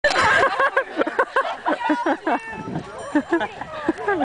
Hit the Butt
Erick attacks a girls butt with a waterballoon.